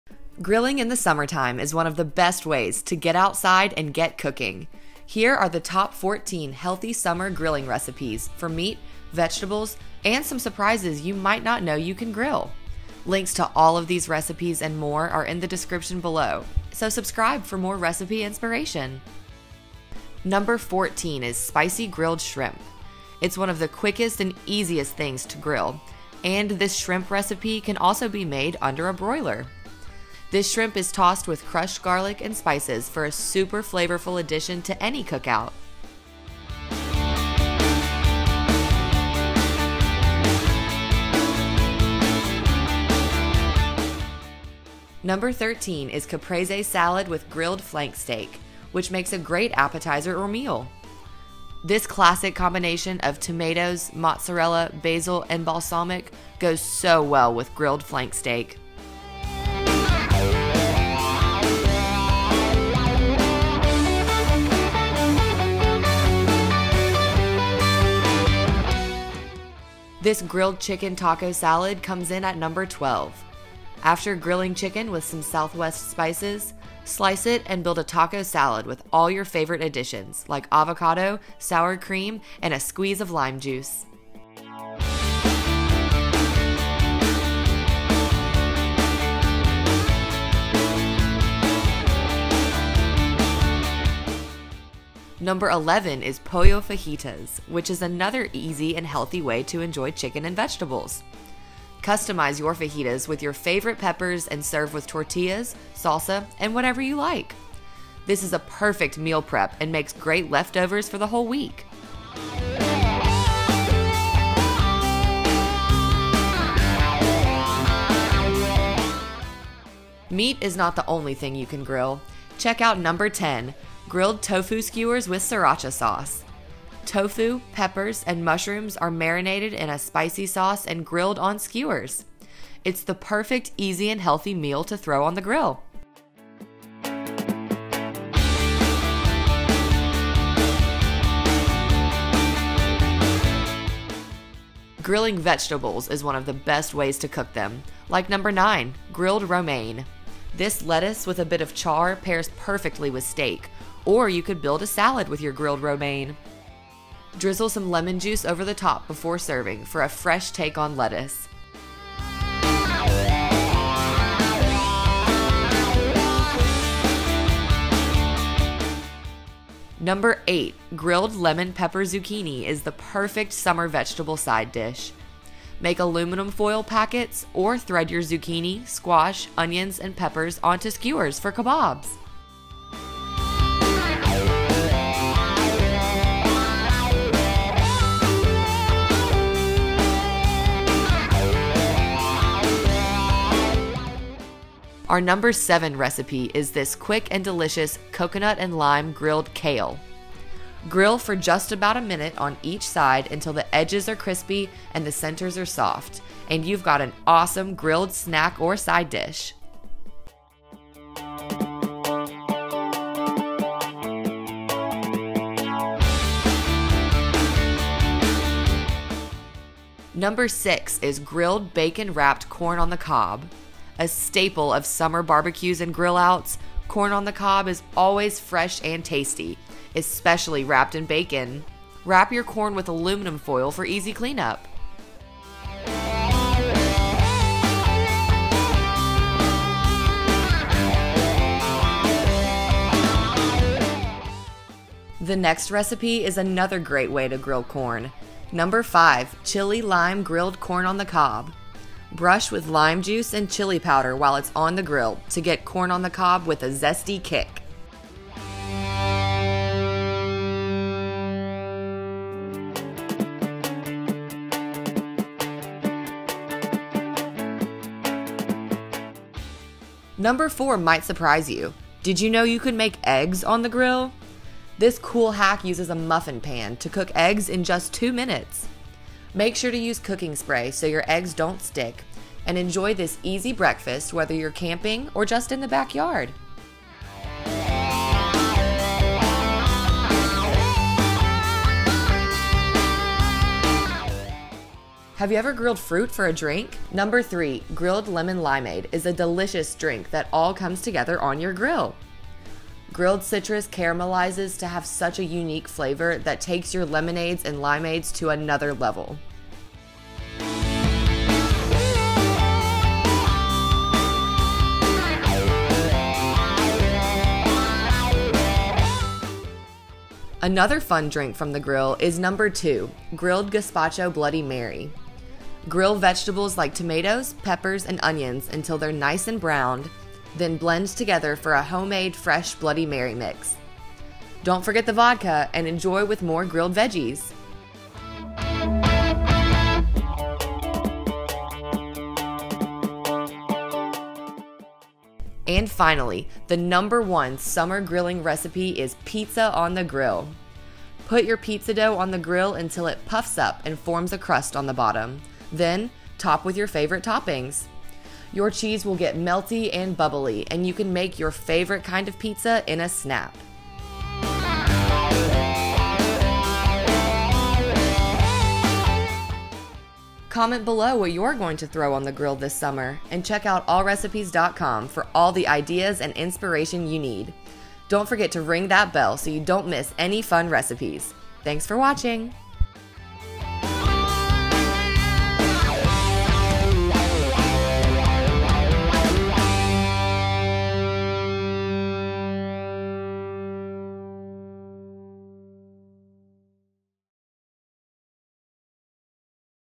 Top 14 Healthy Summer Grilling Recipes | Recipe Compilations | Allrecipes.com
Grilling in the summer is the perfect way to get outside and get cooking! Get the recipes: